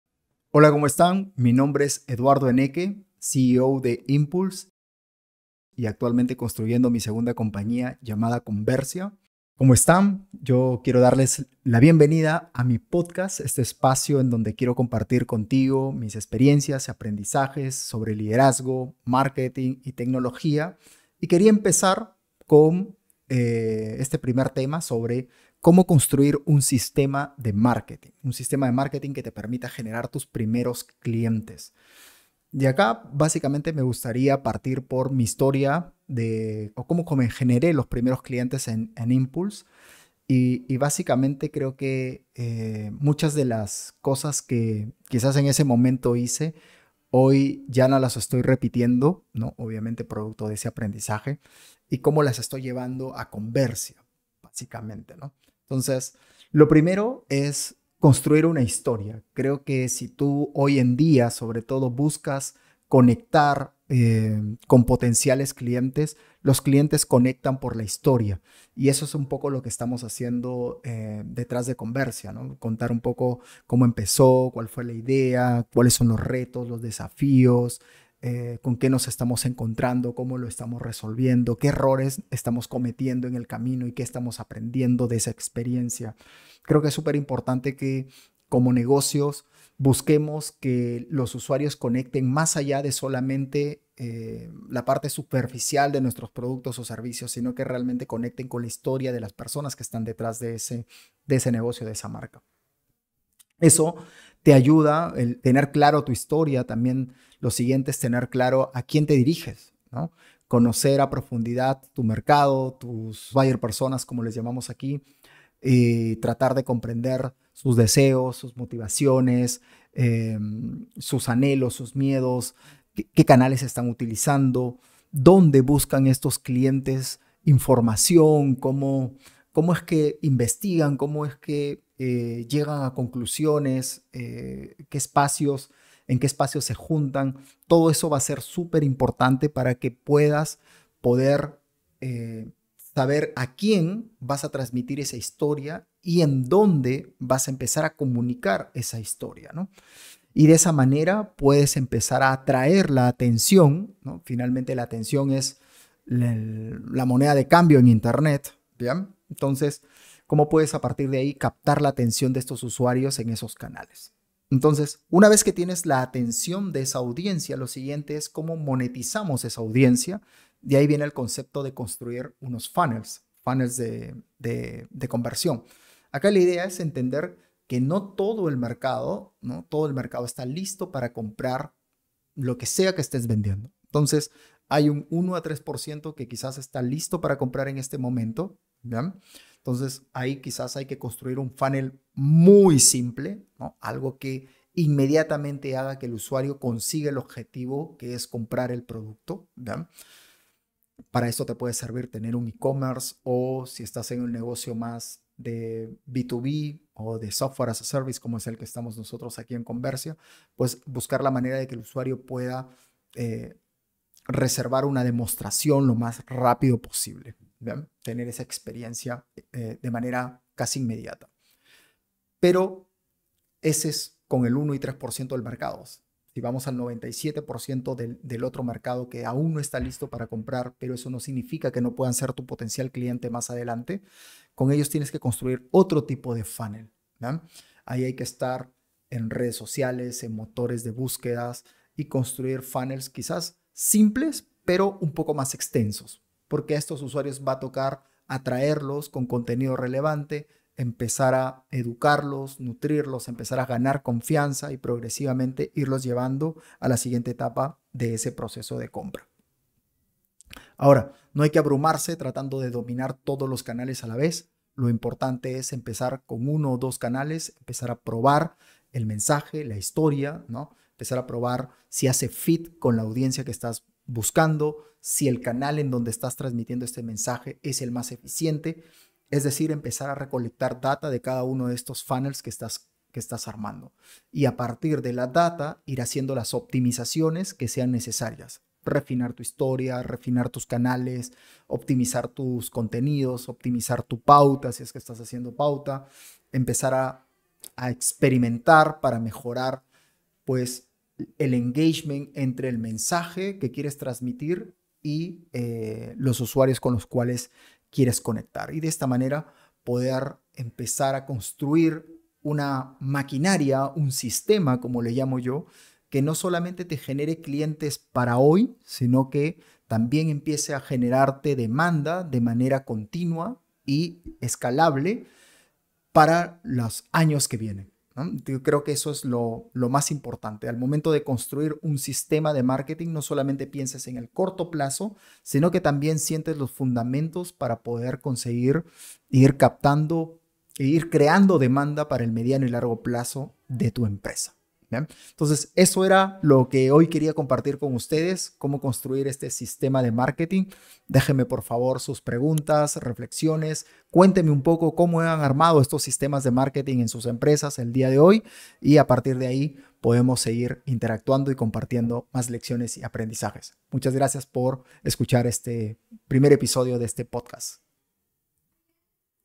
Cómo construir un sistema de adquisición de clientes | EPISODIO 1
🎯 Todos queremos más clientes para nuestro negocio.

Pero la mayoría no tiene un sistema para lograrlo de forma predecible.

En este episodio te comparto 6 pasos que estoy siguiendo para construir el sistema de adquisición de clientes para Conversia, nuestra plataforma de IA Conversacional.

Descubre todos nuestros contenidos sobre Growth, Inbound, HubSpot e IA en nuestro blog